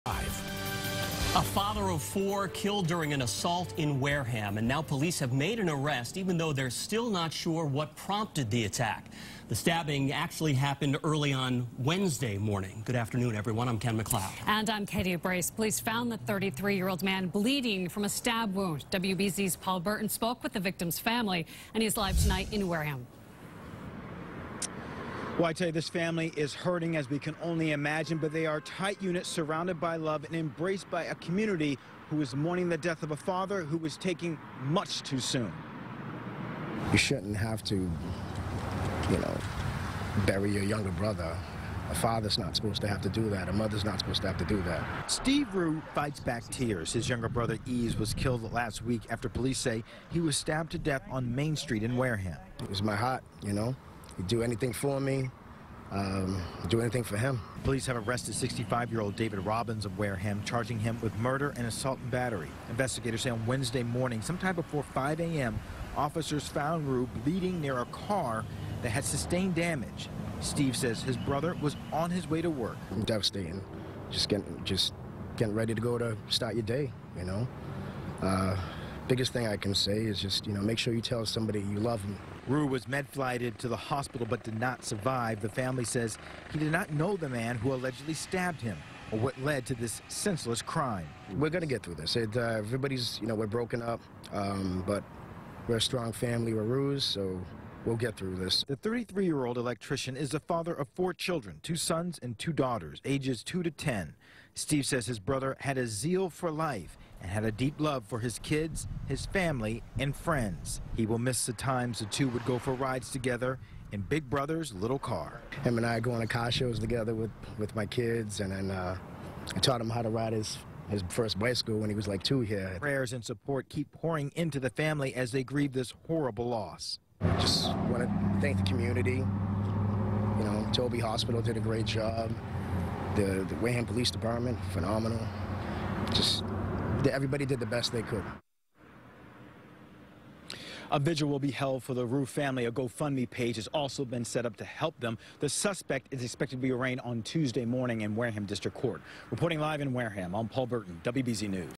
Brother Of Wareham Stabbing Victim Said He Was Caring Father Of Four
A 33-year-old man who died after a stabbing in Wareham on Wednesday is being remembered as a loving and caring father of four. WBZ-TV's Paul Burton reports.